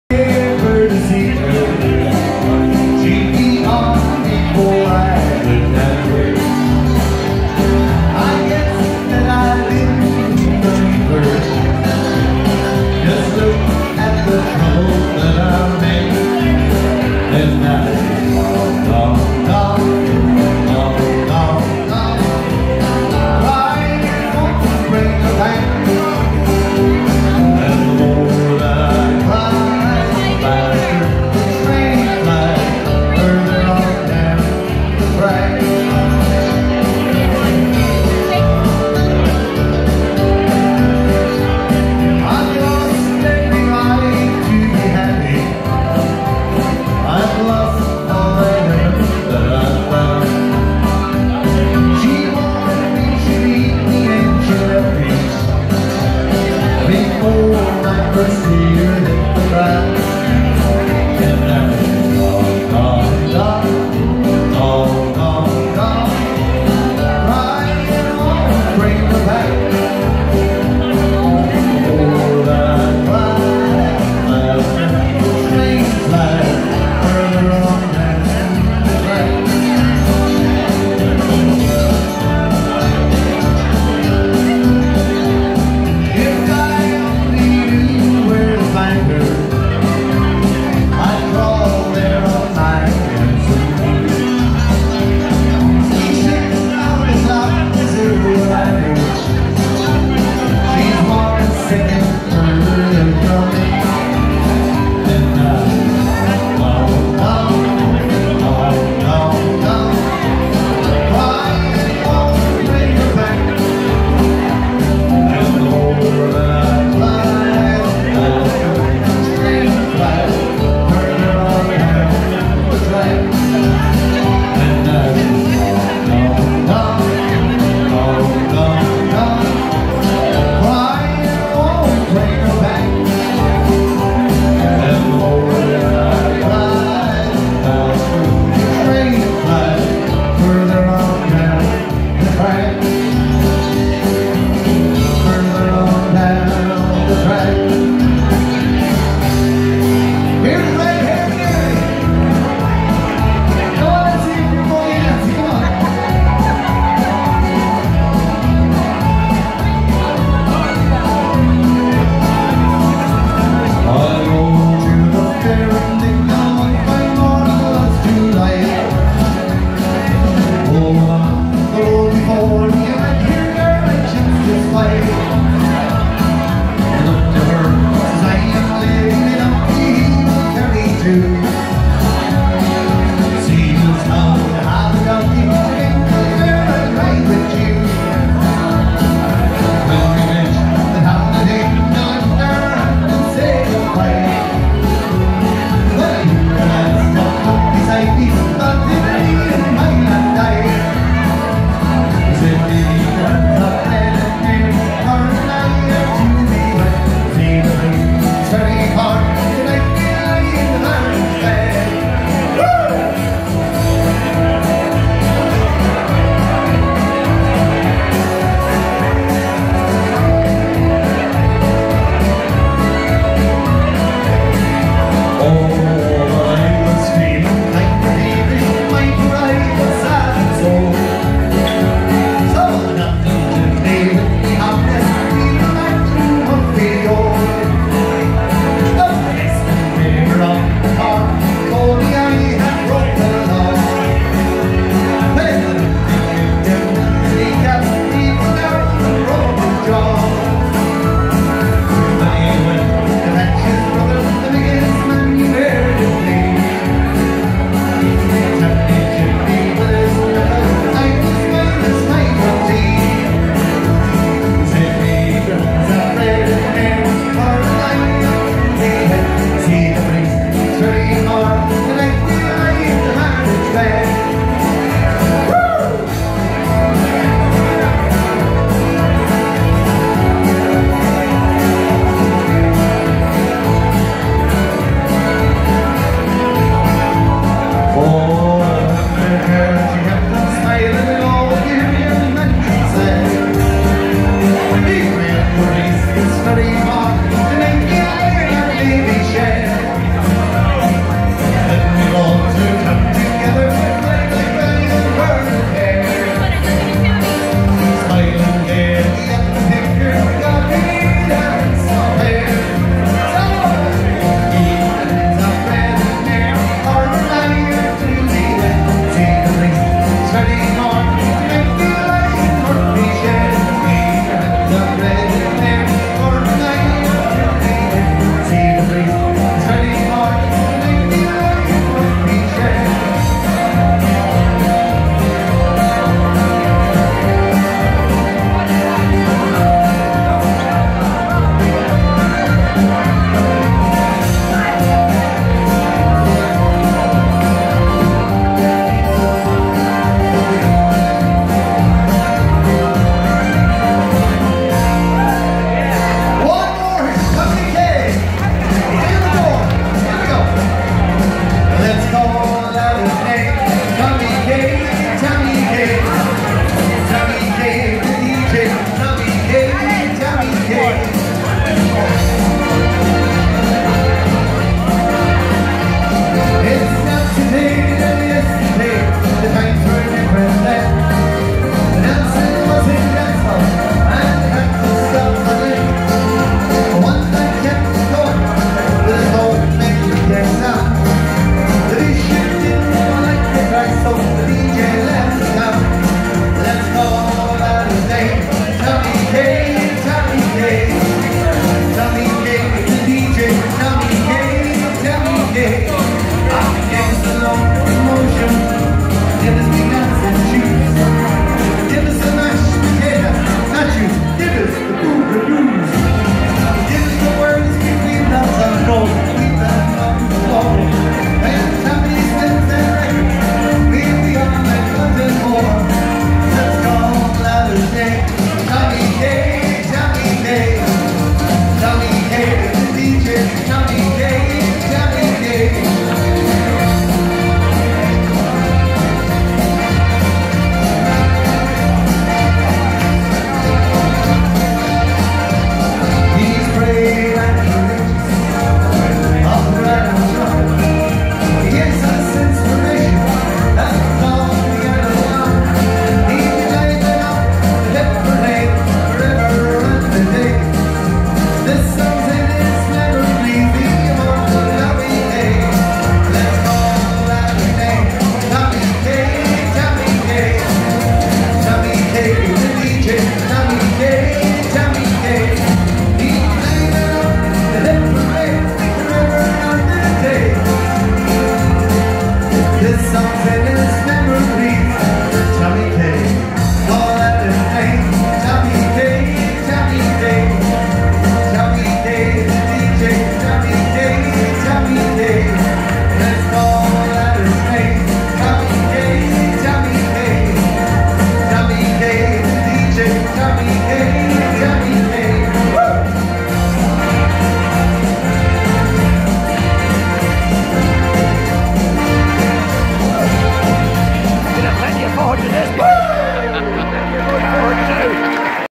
Eddie Quinn singing at the Benefit Night in the Hunterstown Inn - 29th June 2017
Benefit Night in aid of Spina Bifida Hydrocephalus Ireland held in the Hunterstown Inn.

Artists: Matt Leavy, Claire Dillon, Eddie Quinn, Noel Briody, Ray Murtagh, Mickey the Butcher and a host of local guest artists.